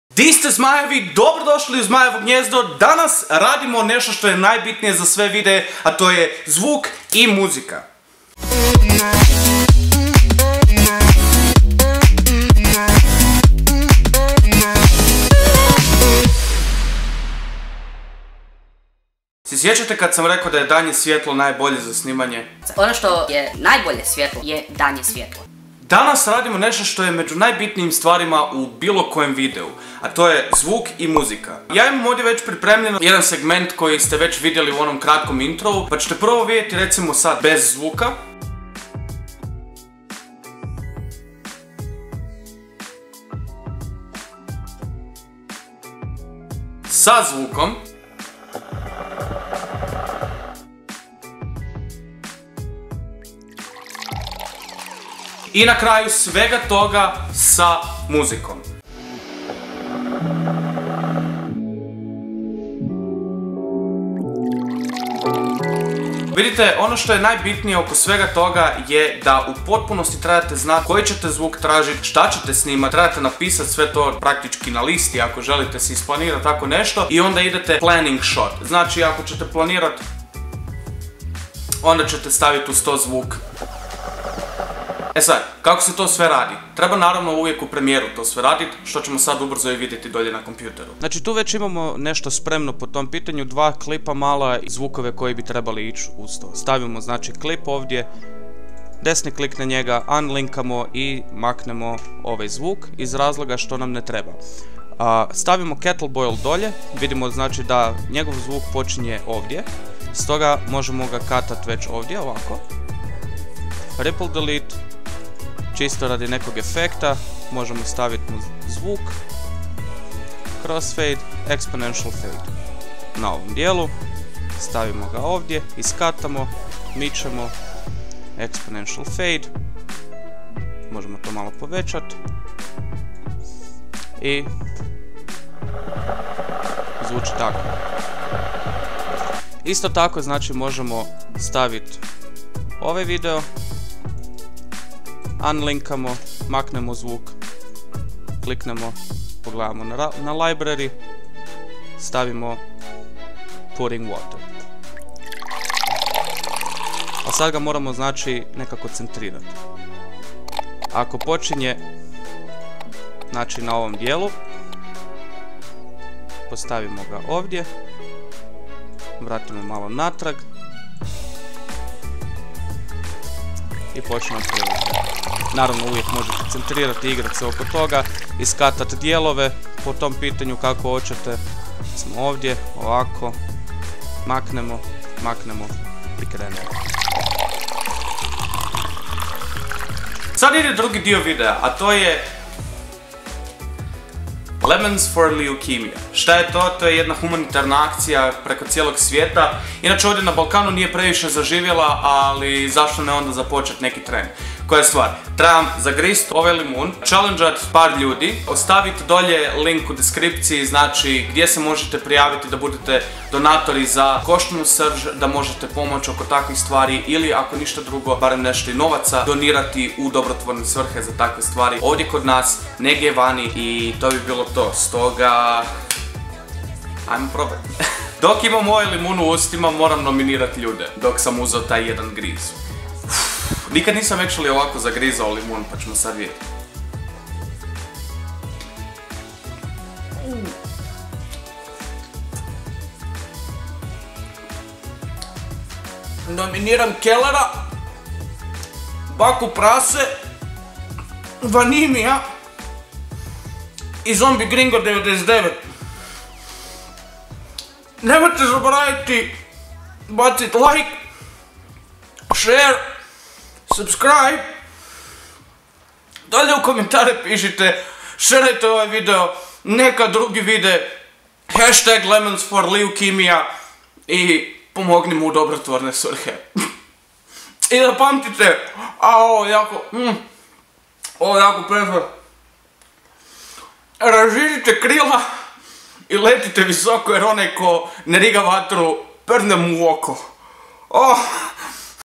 HUMANITARNA AKCIJA LemonsForLeukemia (Tutorial zvuka)
U ovom videu pricamo o vaznosti zvuka i muzike u Vasim videima.

Ali isto i u drugom dijelu radimo Challenge, nominiramo druge youtubere i dizemo svijest o mogucim donacijama za kostanu srz koja pomaze u borbi protiv leukemije!

Raširite krila i letite visoko Zmajevi!

🐲Video je napravljen iz zabave.🐲